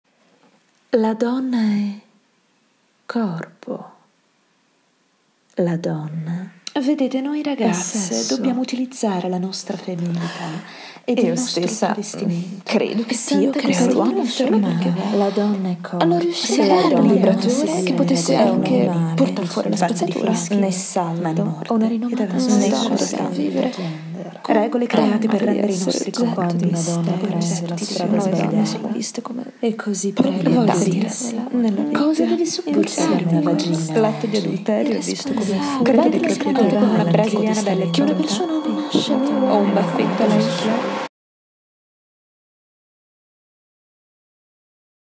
XYX ITA Trailer
Trailer of "XYX" - Italian Edition
Choreography: Moreno Perna
Voice: Giulia Perelli
Camera: Daz Disley and Fenia Kotsopoulou
Filmed at CALA Festival, July 2018, Cori (IT)